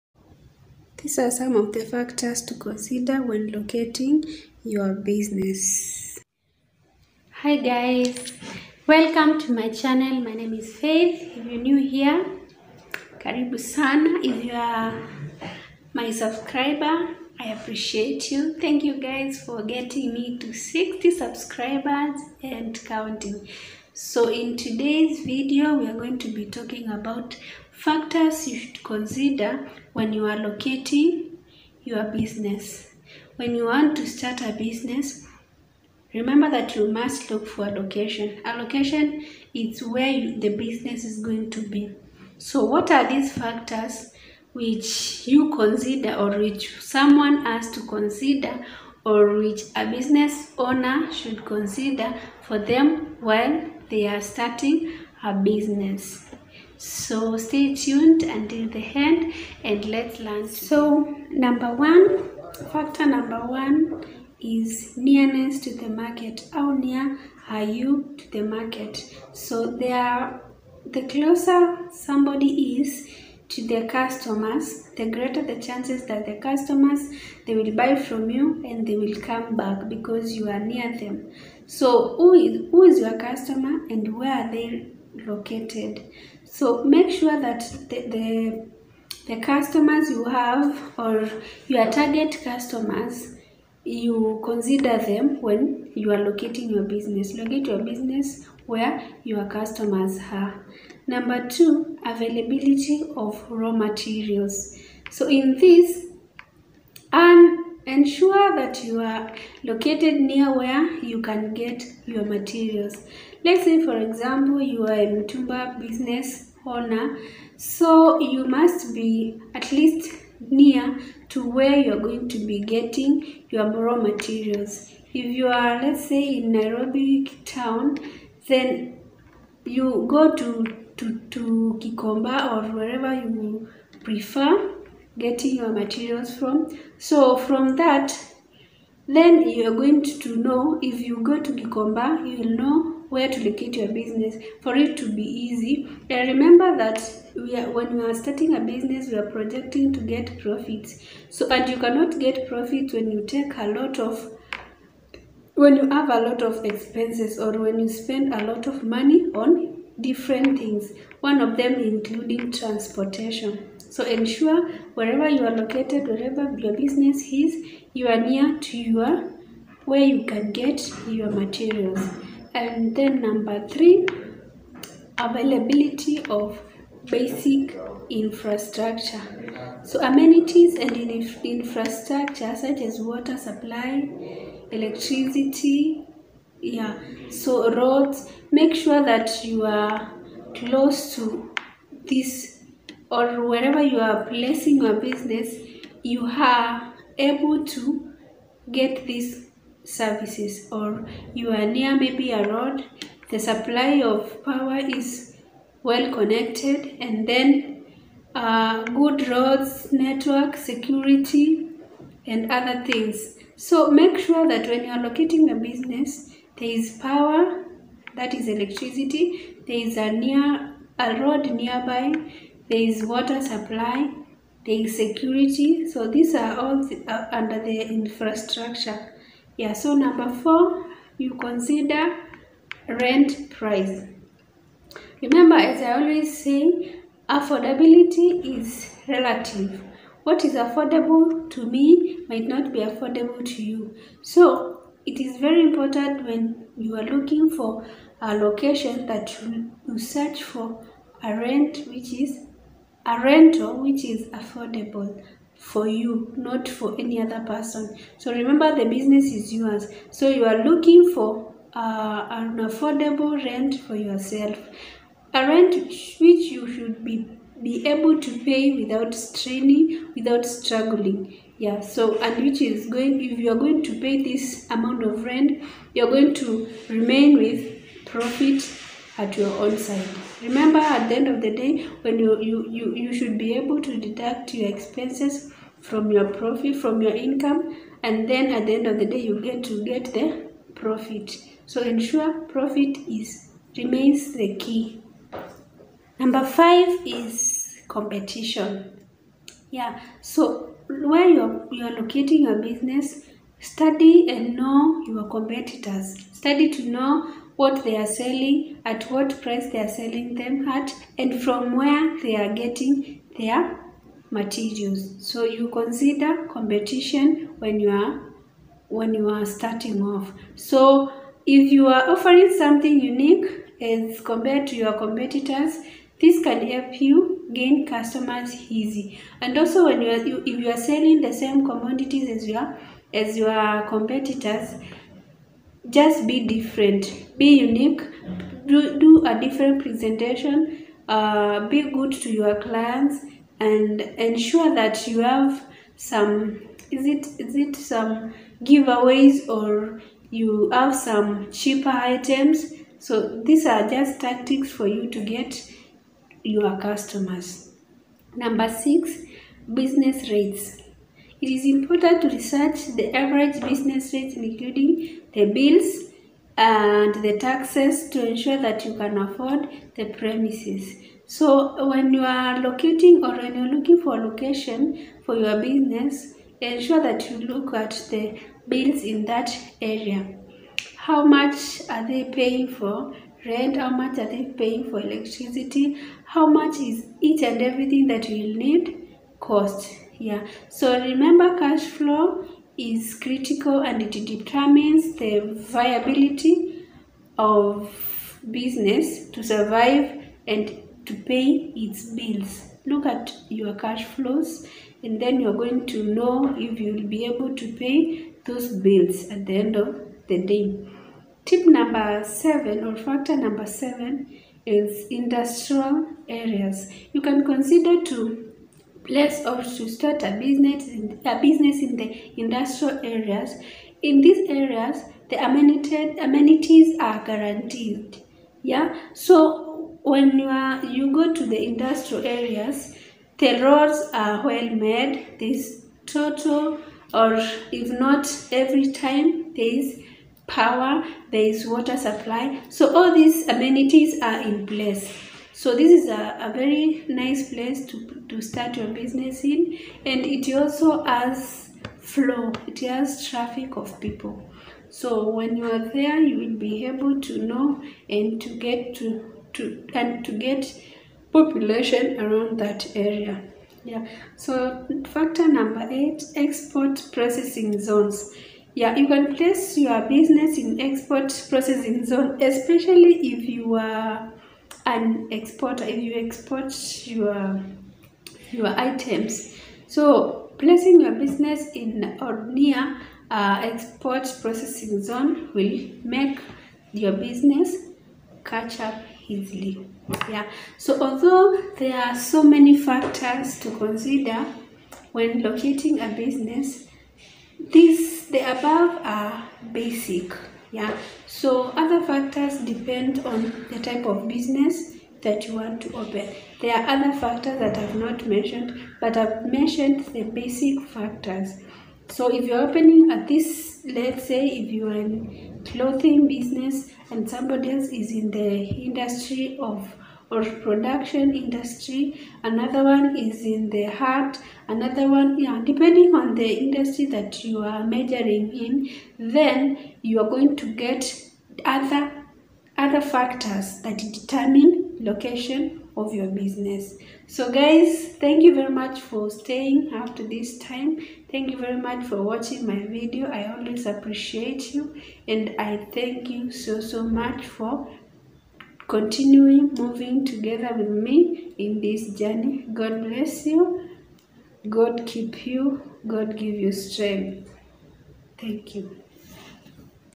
factors to consider when locating a business
find below some factors one can consider when he or she is looking for a location to place a business.